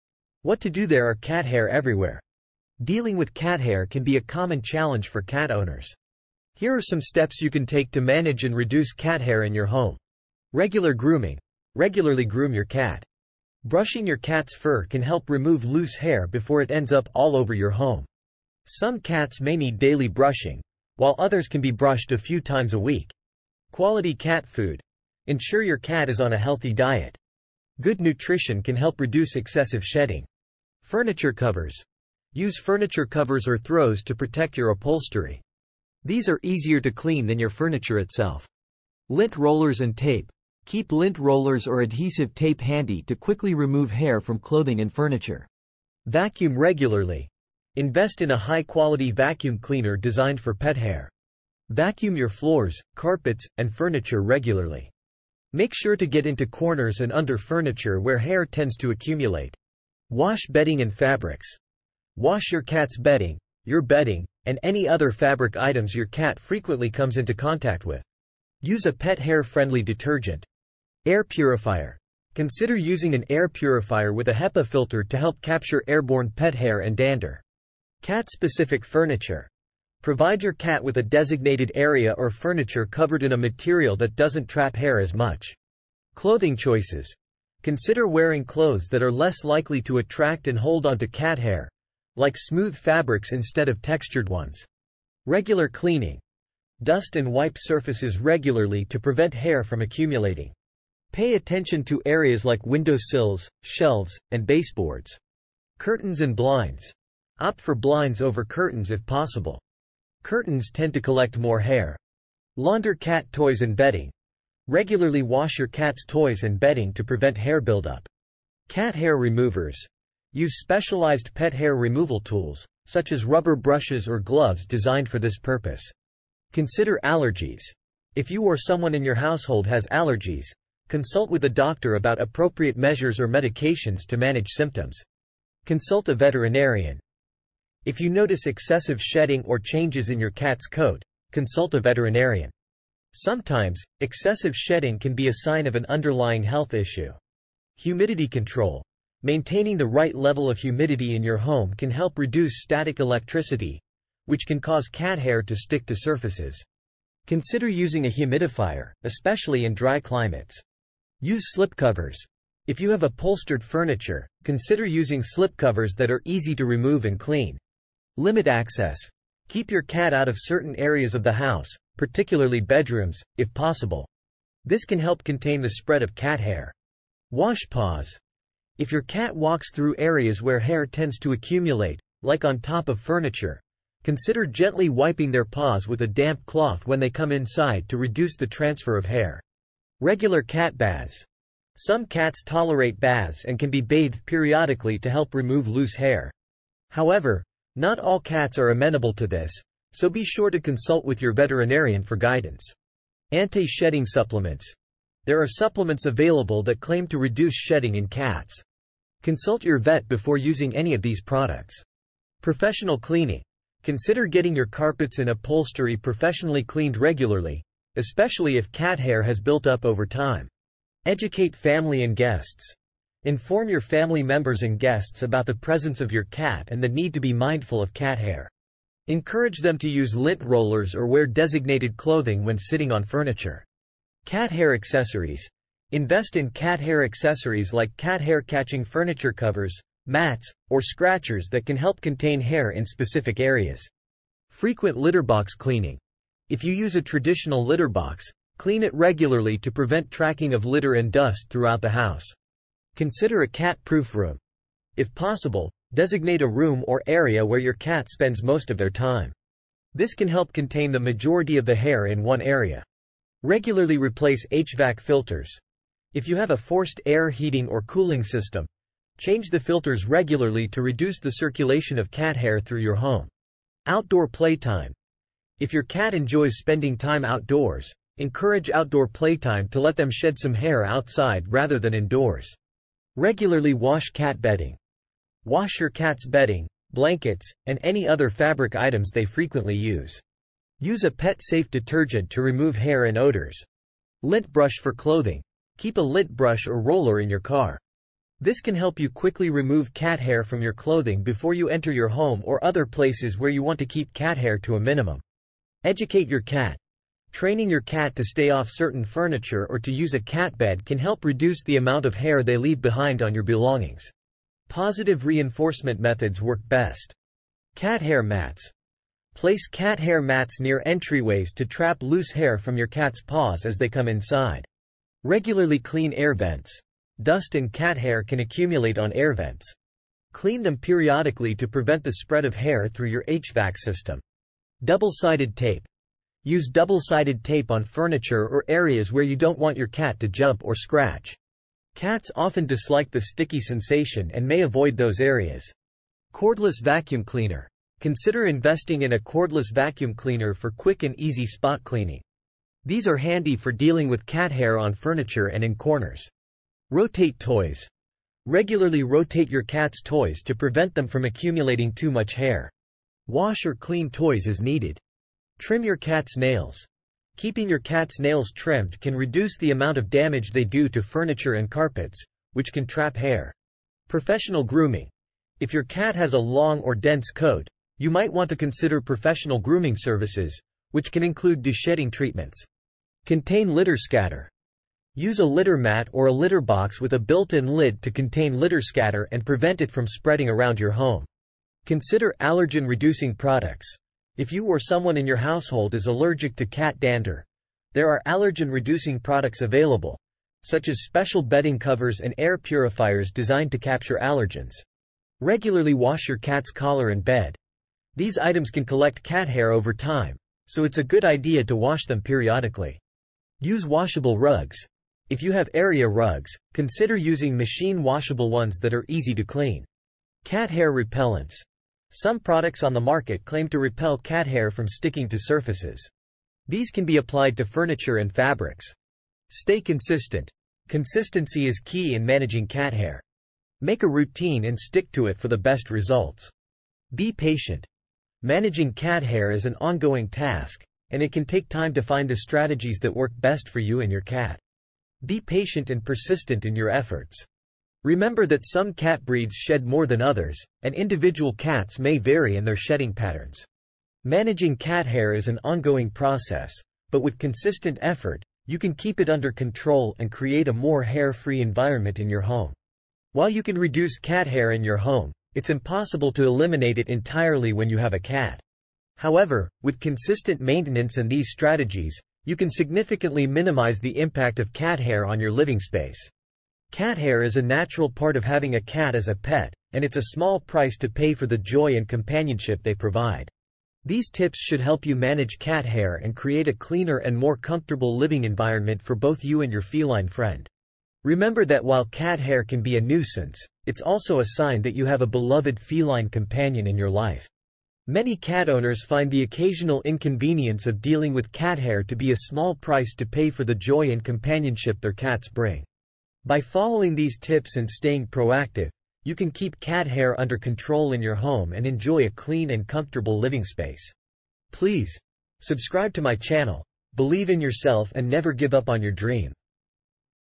What To Do?.. There Are Cat Hair Everywhere.
Cat hair can be a nuisance, it's also a sign that you have a beloved feline companion in your life. Many cat owners find the occasional inconvenience of dealing with cat hair to be a small price to pay for the joy and companionship their cats bring. By following these tips and staying proactive, you can keep cat hair under control in your home and enjoy a clean and comfortable living space.